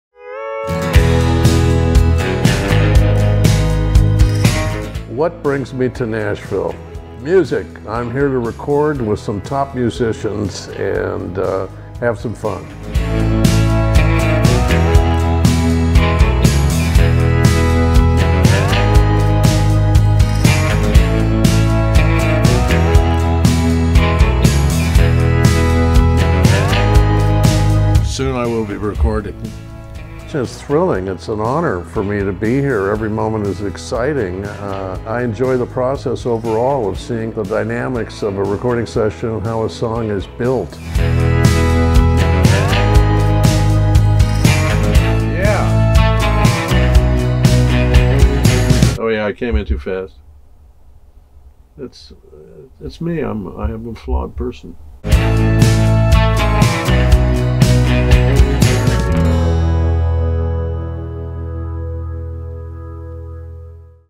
Artist Spotlight: Trevor McShane
🎶 Exciting news! Dive into the magic of our latest music collaboration with legendary producer Don Dixon. 🎸✨ Discover the synergy of Dixon's iconic production style and our unique sound in our upcoming release. 🎧🌟

Directed and Produced By Star Potential Media